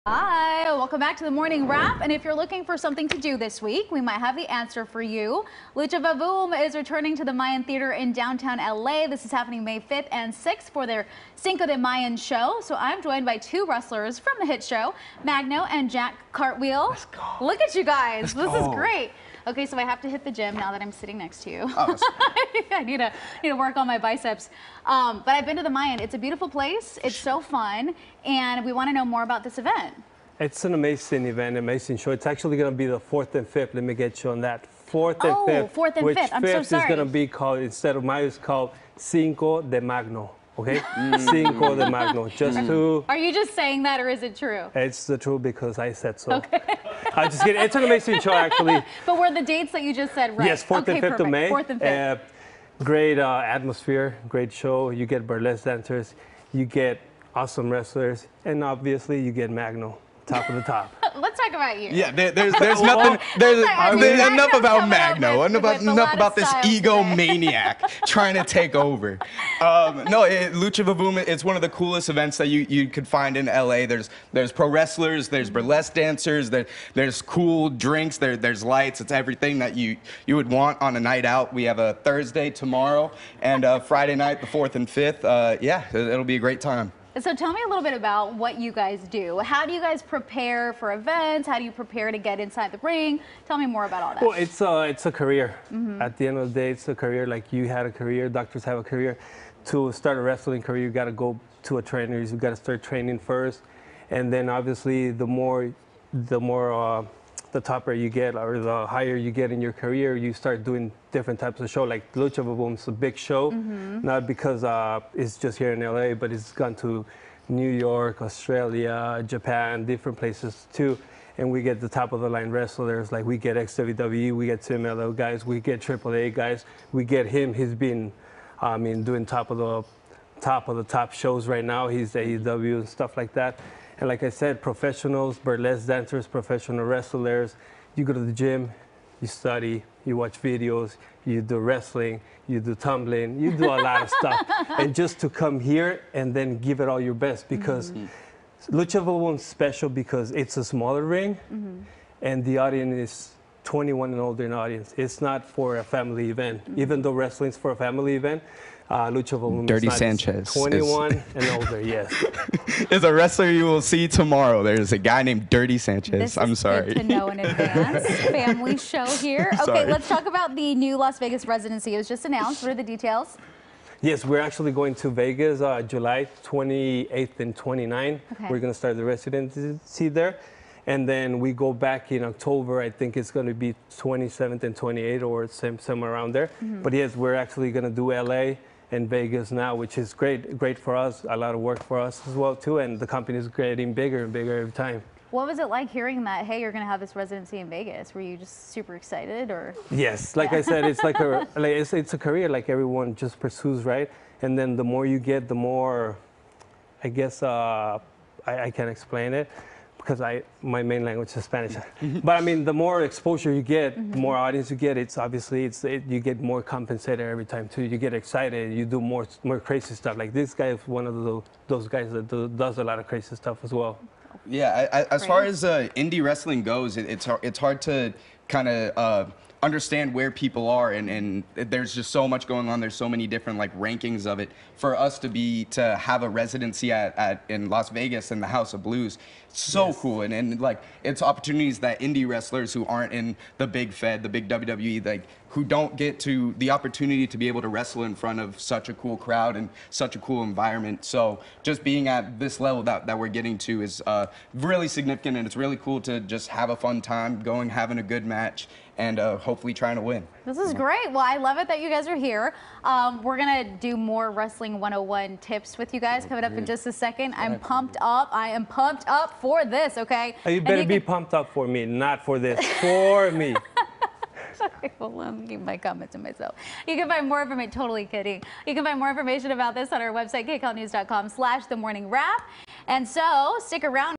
Lucha VaVOOM coming to Los Angeles
Wrestlers Magno “The Man Mountain” Rudo and Jack Cartwheel join us in the studio to talk about Lucha VaVOOM happening on May 4th and 5th.